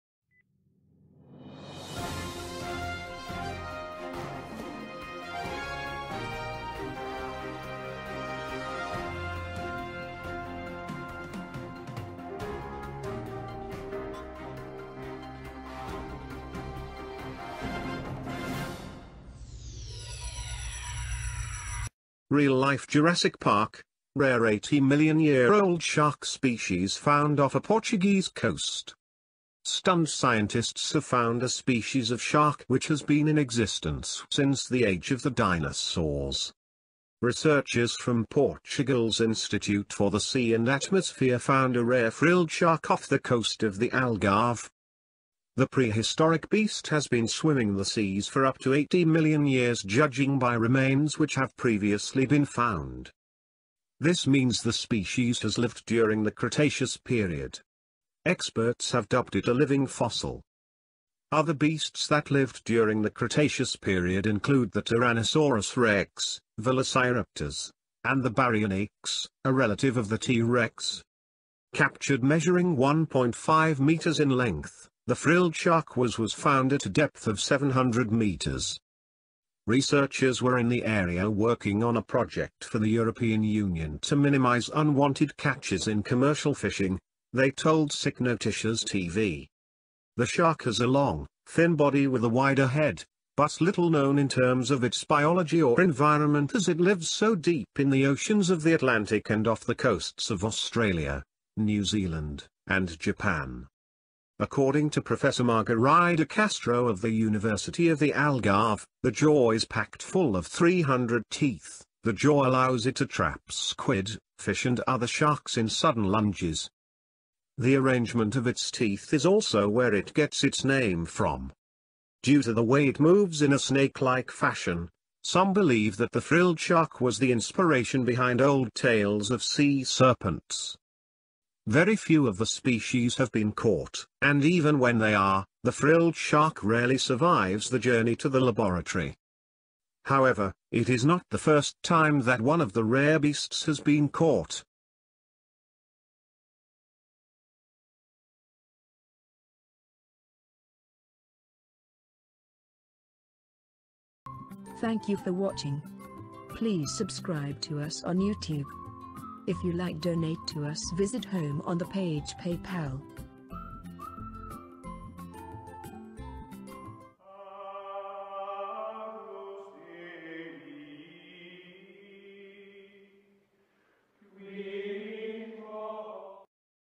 Rare 80 million year old shark species FOUND off Portuguese coast
eal-life Jurassic Park: Rare 80 million-year-old shark species FOUND off Portuguese coast

STUNNED scientists have found a species of shark which has been in existence since the age of the dinosaurs.

Researchers from Portugal’s Institute for the Sea and Atmosphere found a rare frilled shark off the coast of the Algarve.

The prehistoric beast has been swimming the seas for up to 80 million years judging by remains which have previously been found.

This means the species has lived during the cretaceous period. Experts have dubbed it a “living fossil”.